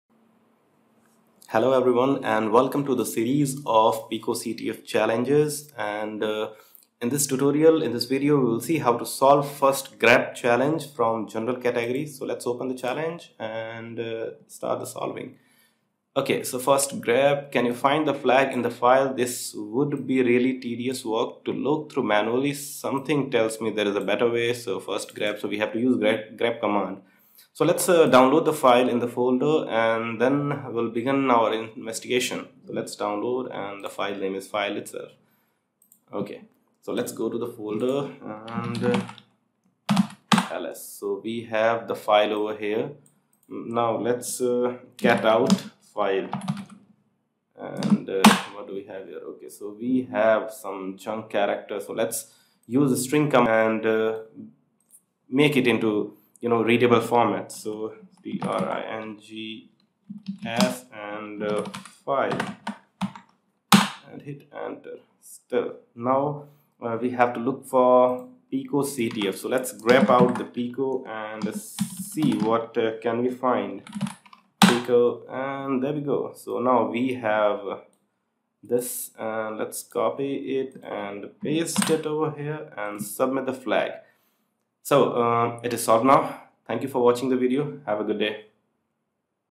First Grep |picoCTF challange |walkthrough
This video is a walkthrough of "First Grep " challange from pico CTF "general skills" Category.